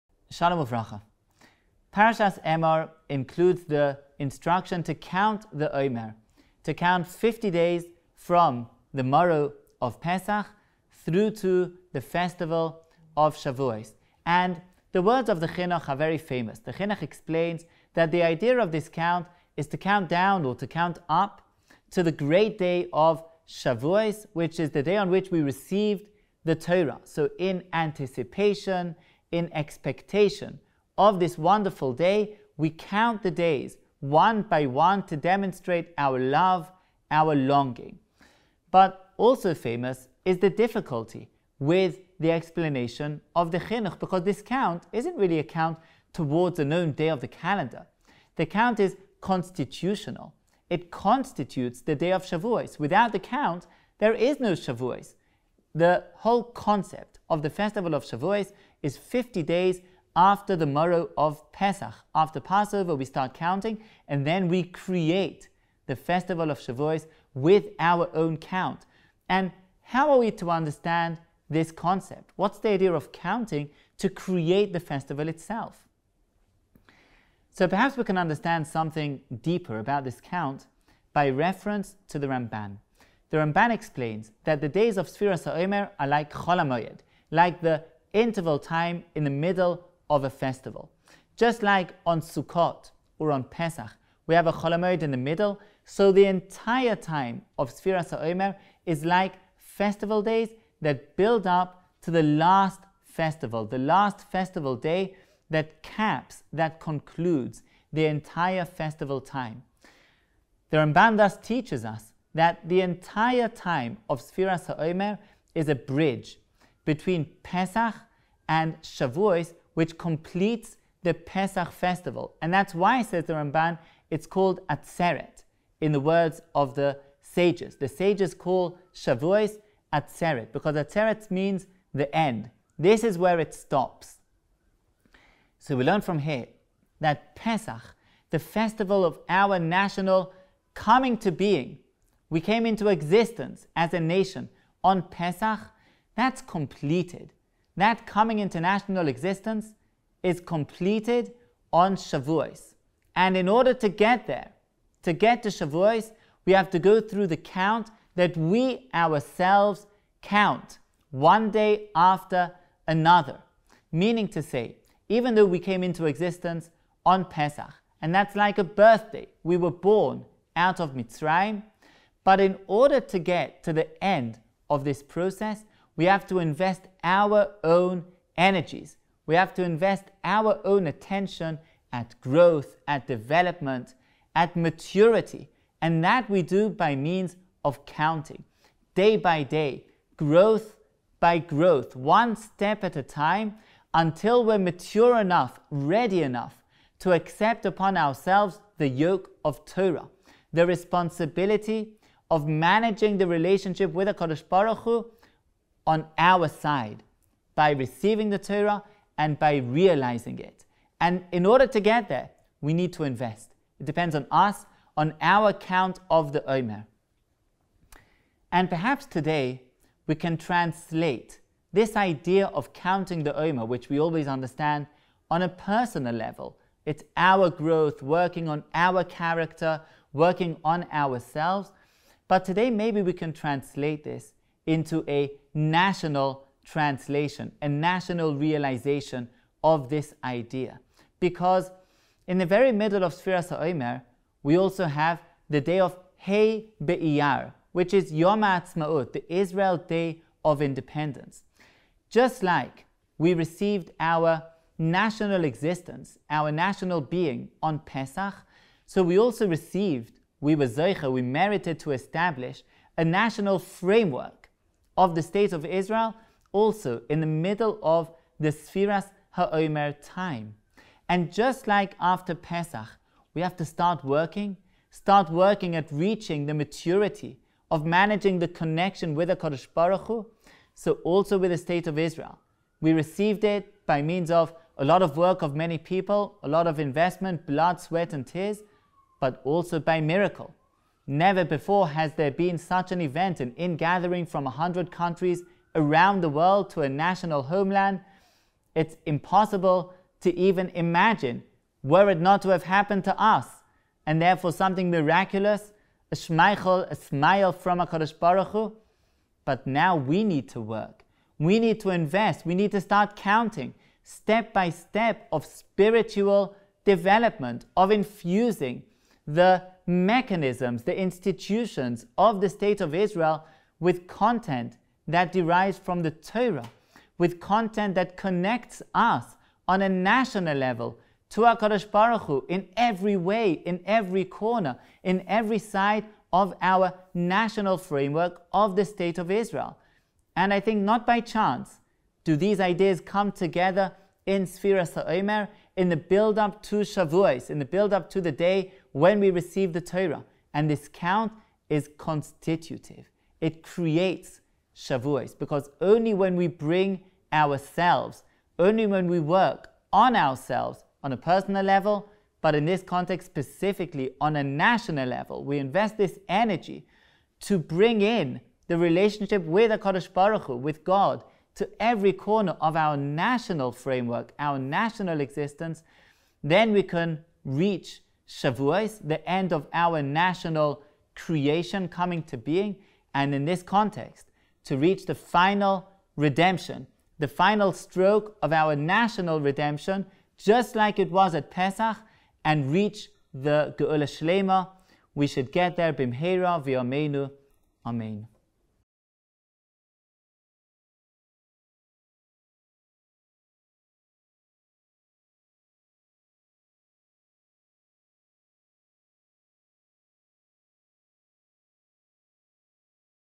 The National Sefira Count | Parashas Emor | Rabbi Yehoshua Pfeffer
Sefiras Ha'Omer is a period of growth and development that constitutes Shavuos. Today, in the State of Israel era, we are called to count even in the national sense.

Sefiras Ha'Ome